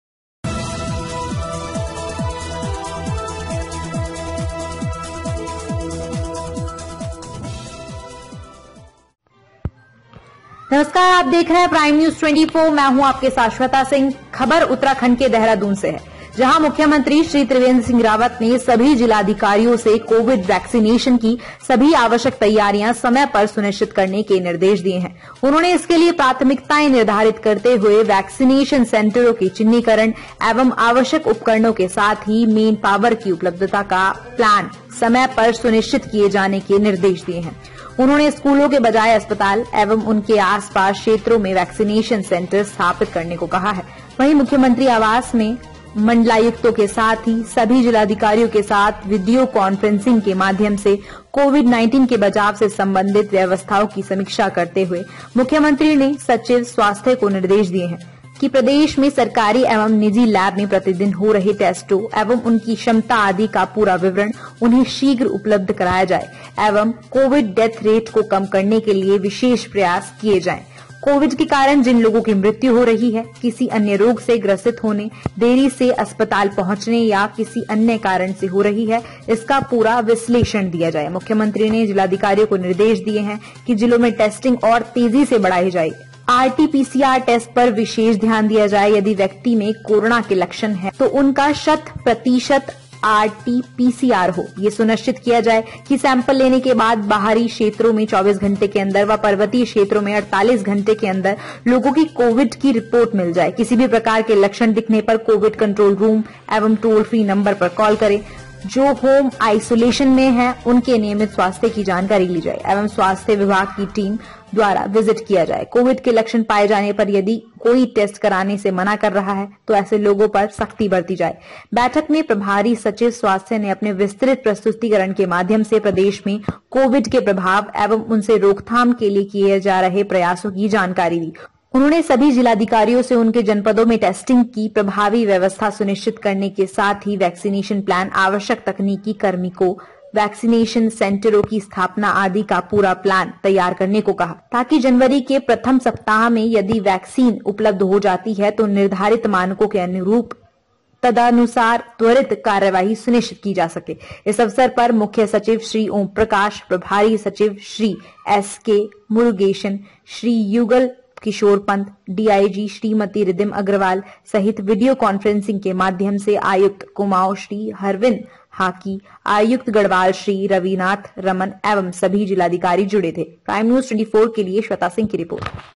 जिलाधिकारियों से कोविड वेक्सिनेशन की सभी आवश्यक तैयारियां समय पर सुनिश्चित करने के निर्देश दिये है
सभी जिलाधिकारियों से कोविड वेक्सिनेशन की सभी आवश्यक तैयारियां समय पर सुनिश्चित करने के निर्देश दिये है